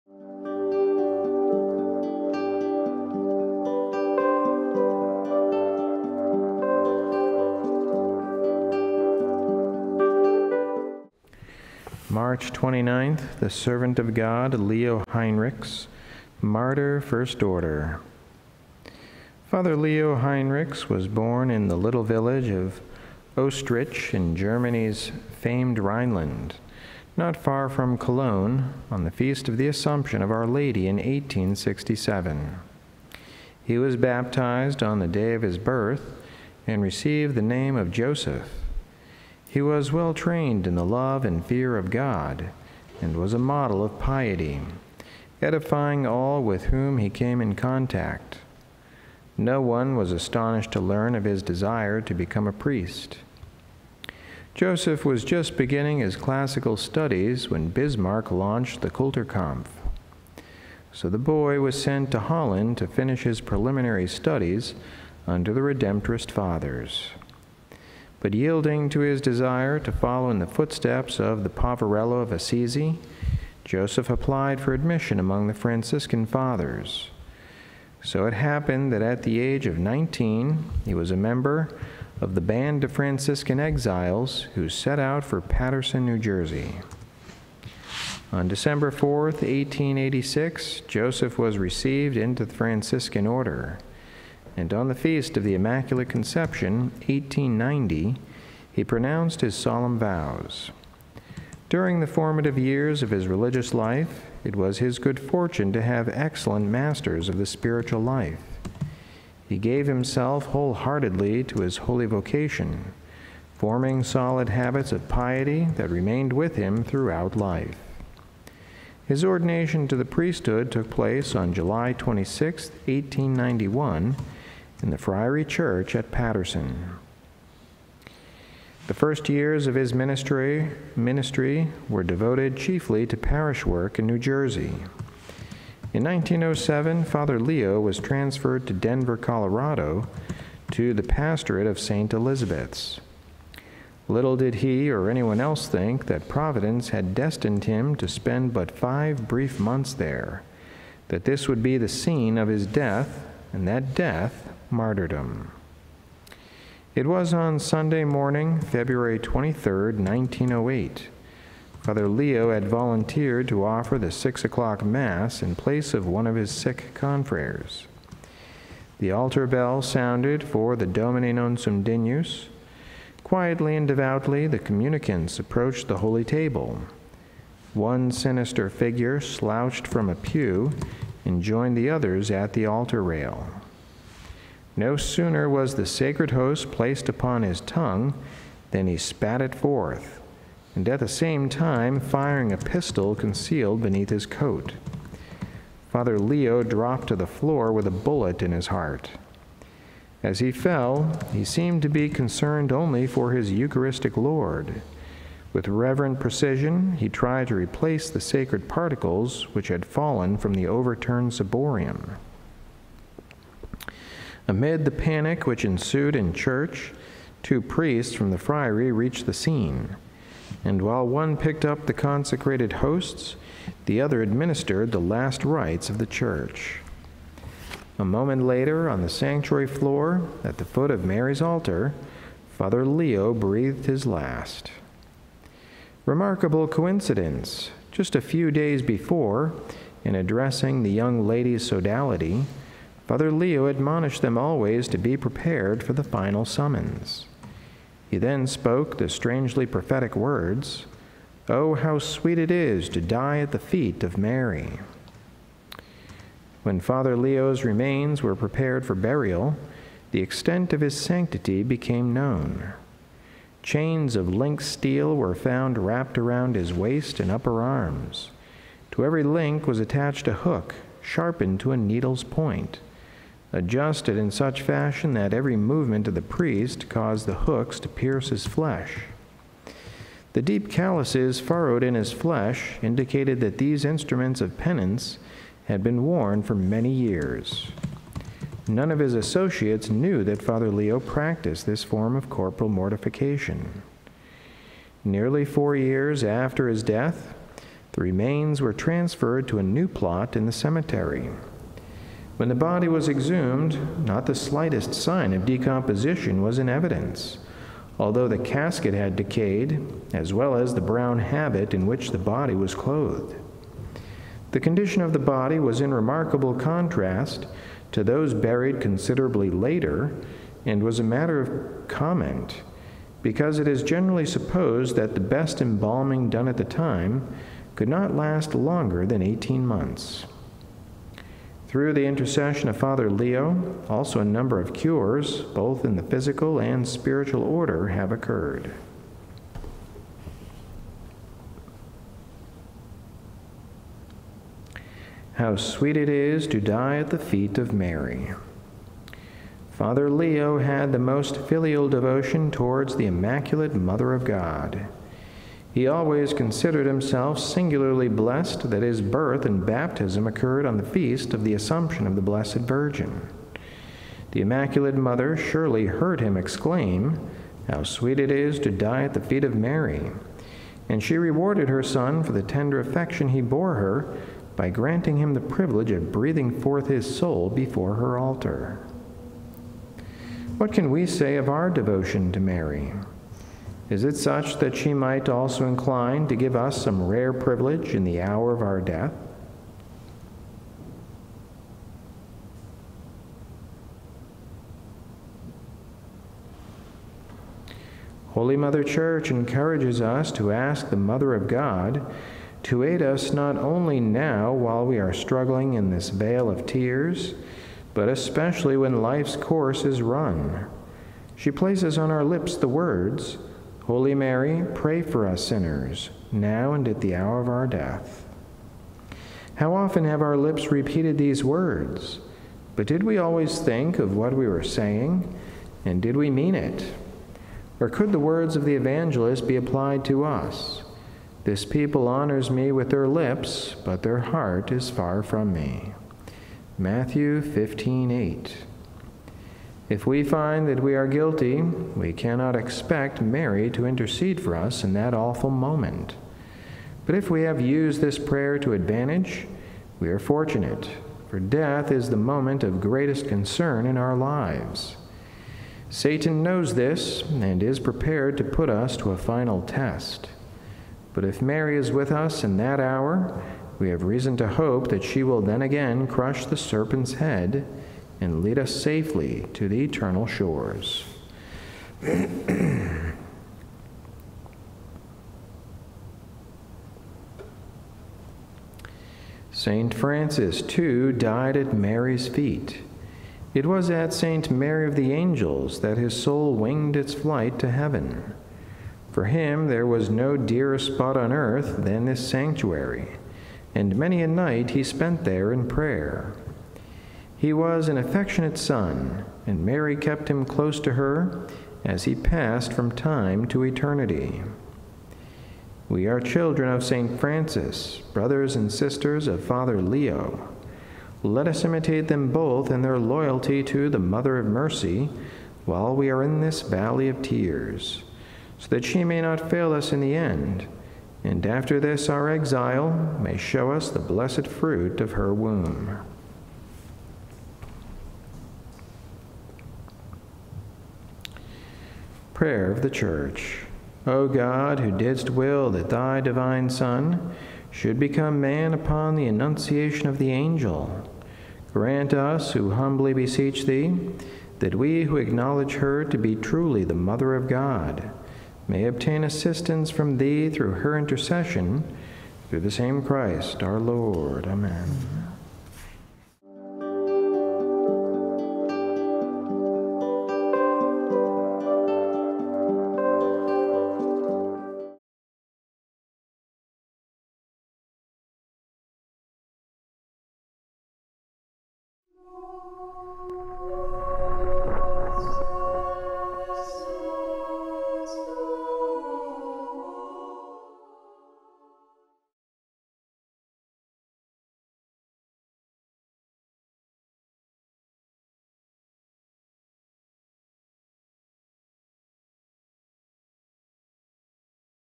Mar 29 - Serv. of God Leo Heinrichs - Franciscan Saint of the Day
The Life of Serv. of God Leo Heinrichs, Martyr, First Order (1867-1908) for the Franciscan Saint of the Day for Mar 29. Leo Heinrichs was born in Germany but immigrated to the United States with his exiled confreres. While at mass in Denver, Colorado he was killed by a man with a gun.

Reflection is on How sweet to die at the feet of Mary
our heart must follow our lips
she will be there at the hour of death
St Francis died at her feet at St Mary of the Angels

Read by Fr. Ignatius Manfredonia, FI from Marion Habig's 'The Franciscan Book of Saints' Revised Edition, Franciscan Herald Press, ISBN: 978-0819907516

Ave Maria!